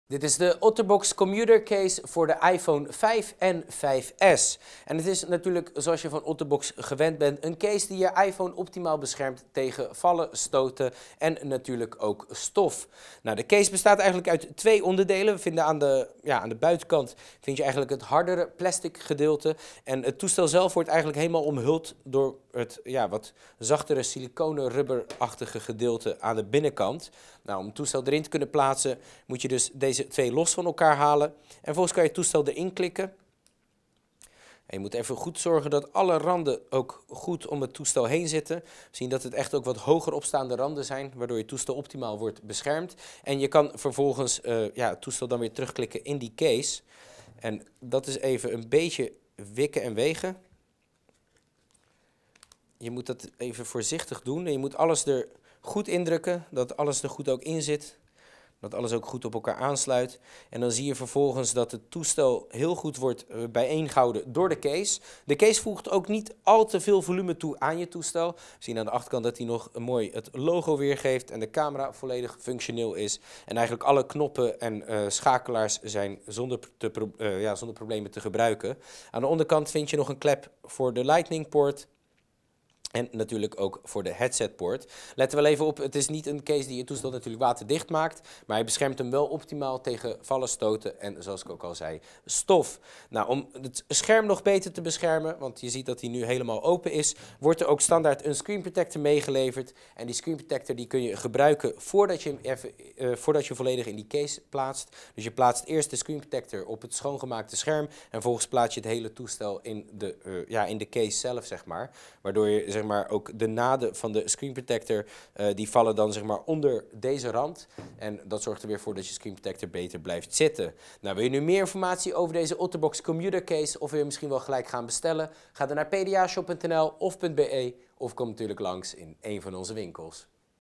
OtterBox Commuter Case Apple iPhone 5/5S Productvideo (NL/BE)
Meer informatie of bestellen? Ga naar

De Otterbox Commuter combineert de stevigheid van de Otterbox Defender cases en de zachte siliconen afwerking van de Otterbox Impact cases in een slanke body. Hiermee is de Otterbox Commuter dè case voor wie uitstekende bescherming verwacht zonder het uiterlijk van de Apple iPhone 5 / 5S teveel aan te passen. Dankzij de handige openingen zijn de aansluitingen van jouw iPhone 5 / 5S daarnaast gewoon bereikbaar wanneer deze zich in de case bevindt. De Otterbox Commuter wordt geleverd inclusief een microfiber-doekje en een screenprotector.